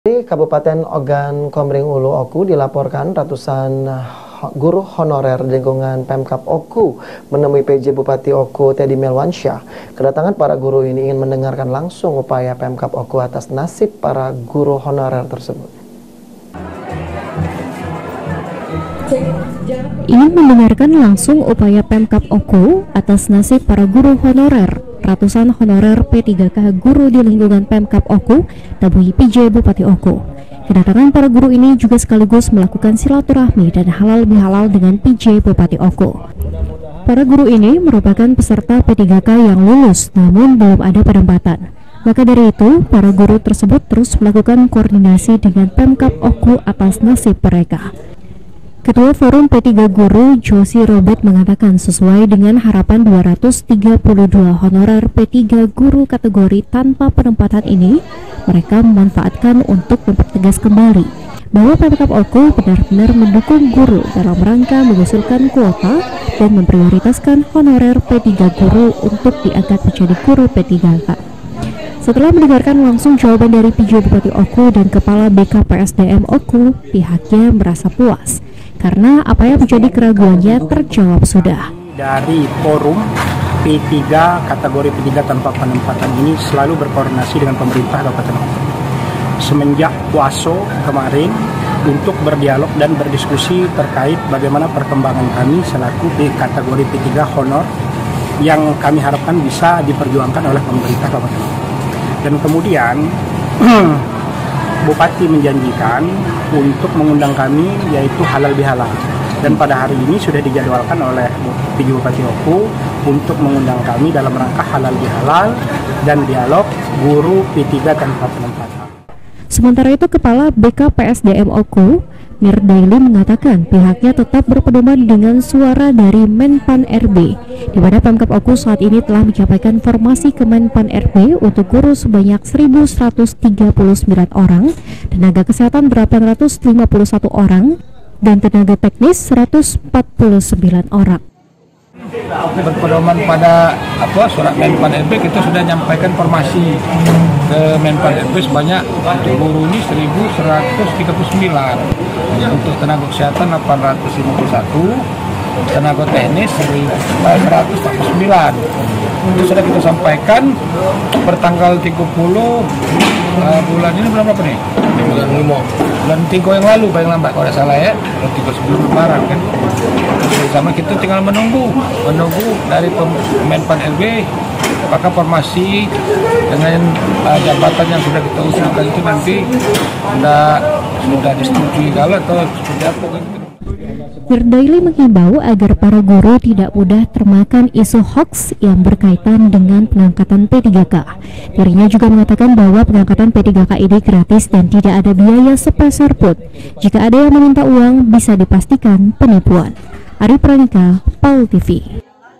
GURU HONORER KEMBALI TANYAKAN NASIB PENGANGKATAN | PALTV
Ratusan Honorer P3k Guru Dilingkungan Pemkab Oku Temui Pj Bupati Oku// Kedatangan Para Guru Ini Ingin Mendengarkan Langsung Upaya Pemkab Oku Atas Nasib Para Guru Honorer///